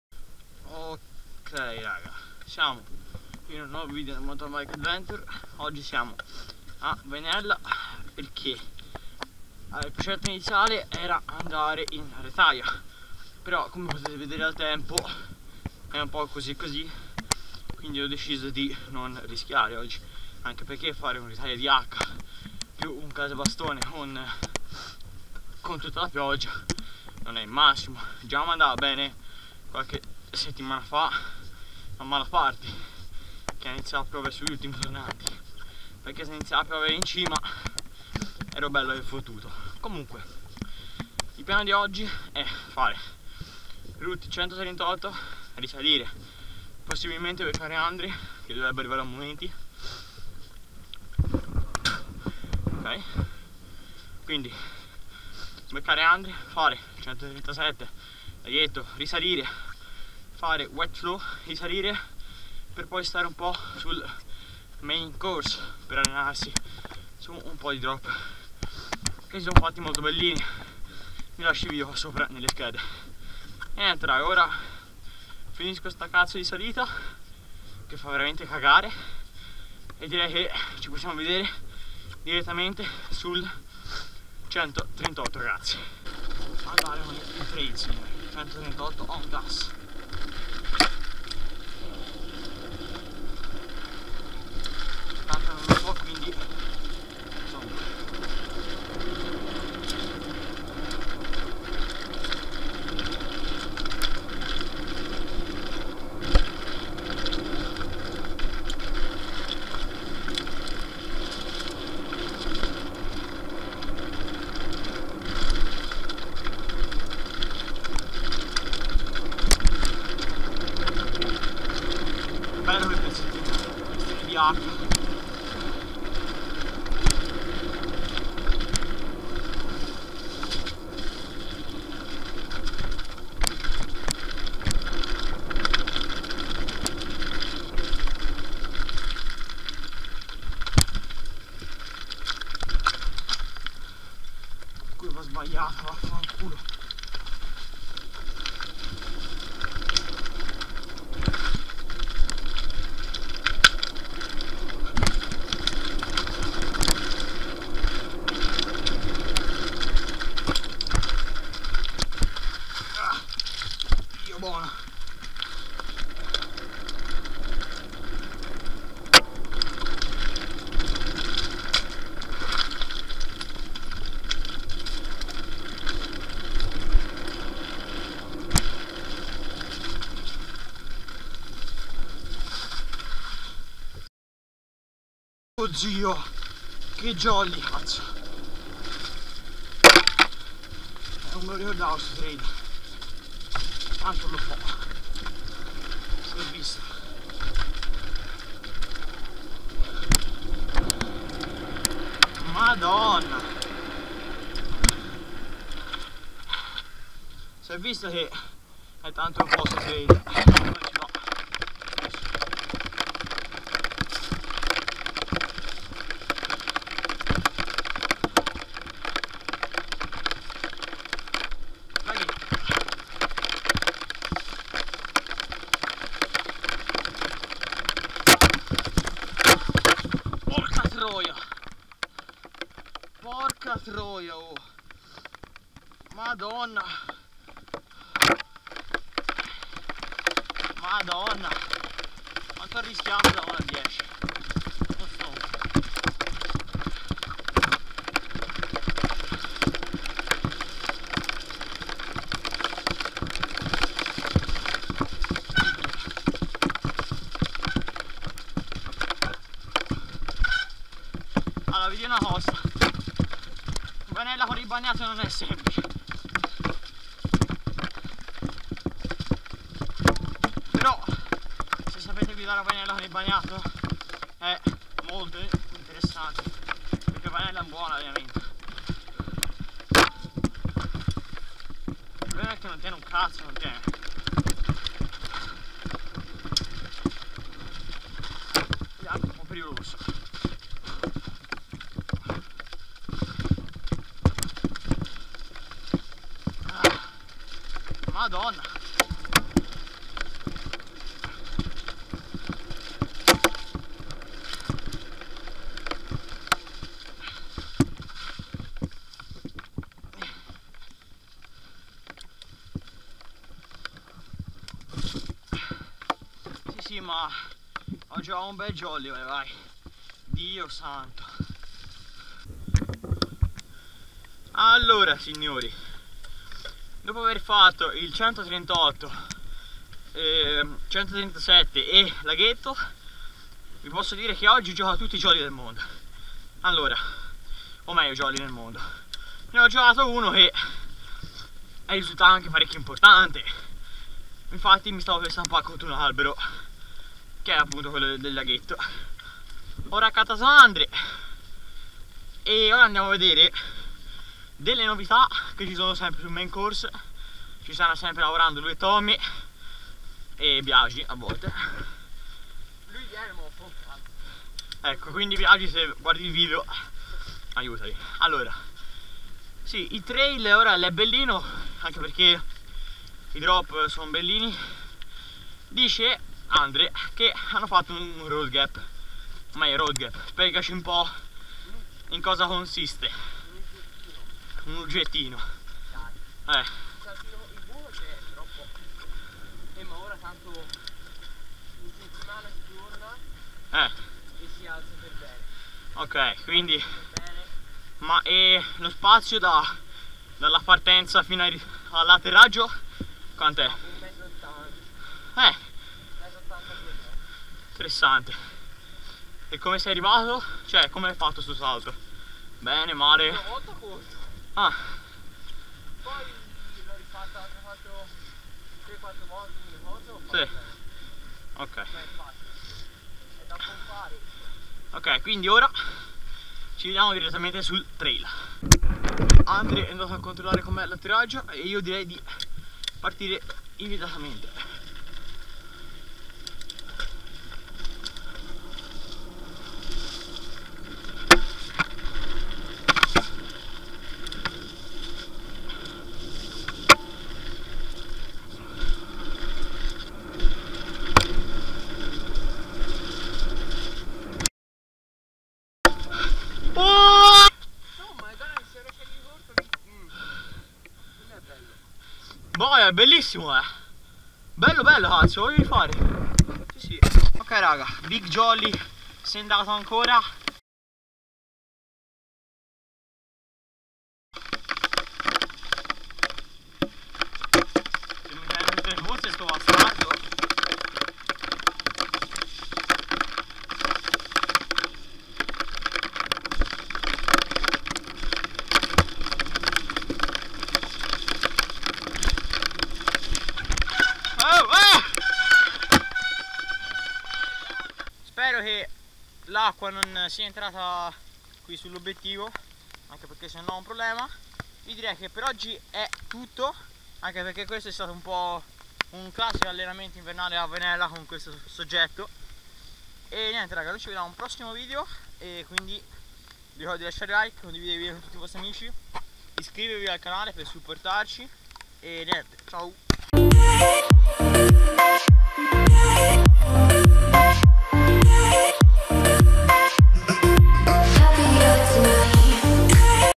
UN CLASSICO WEEKEND INVERNALE A VAINELLA
Salve ragazzi, in questo video vi faccio vedere un classico weekend invernale a vainella con tanto di pioggia!

Link Trail:
Route 138:

MY BIKE: lapierre zesty tr 4.9
Custom: